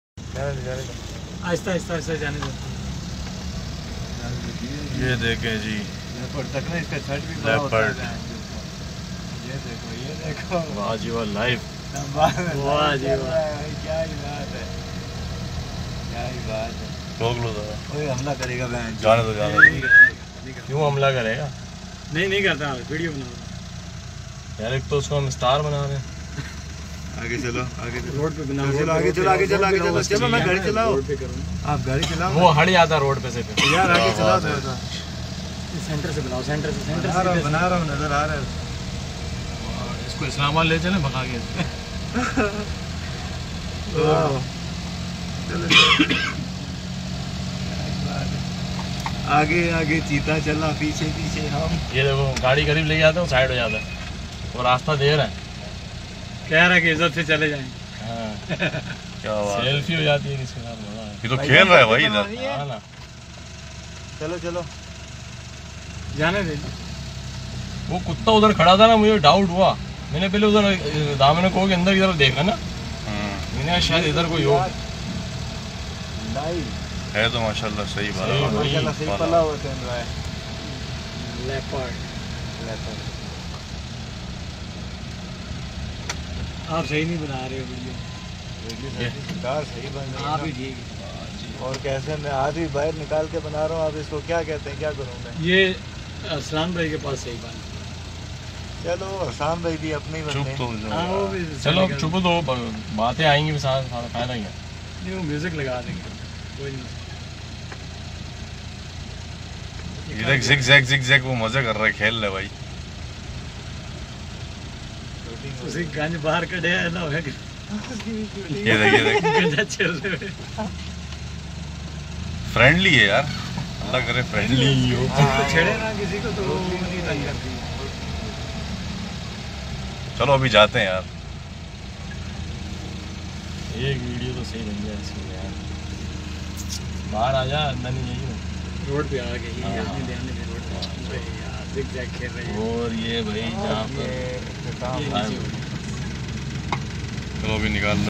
Snow Leapord on the Streets of Islamabad - Pir Sohawa 26.10.2020
This is realtime footage of a snow leapord walking on the streets of Islamabad. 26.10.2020